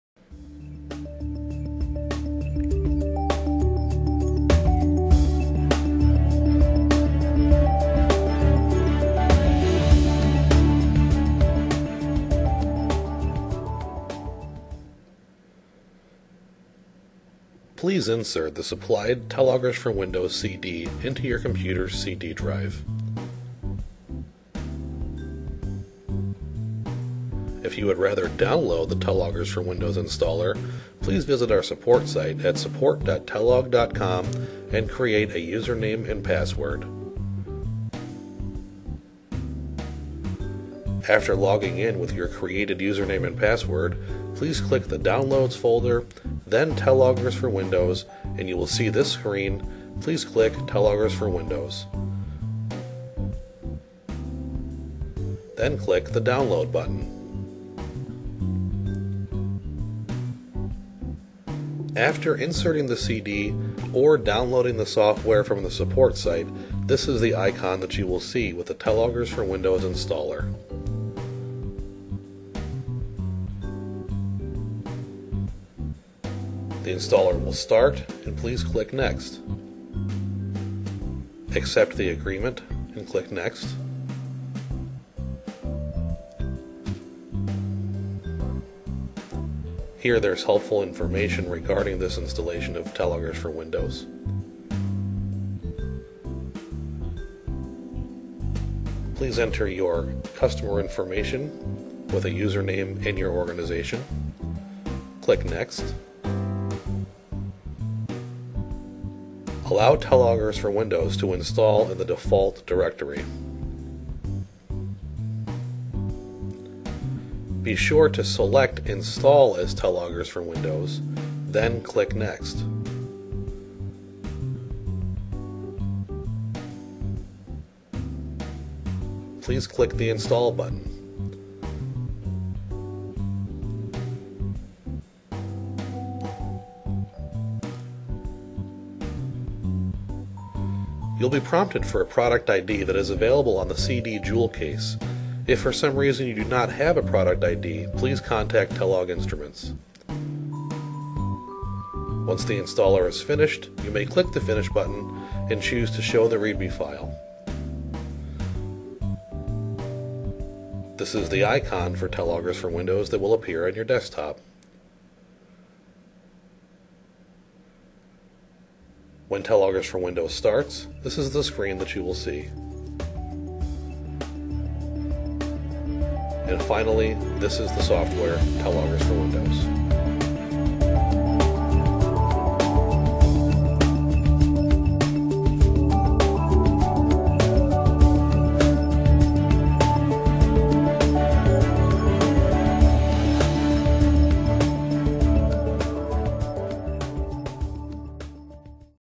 Installing Telogers for Windows
The video will assist customers with installing Telogers for Windows on their computer.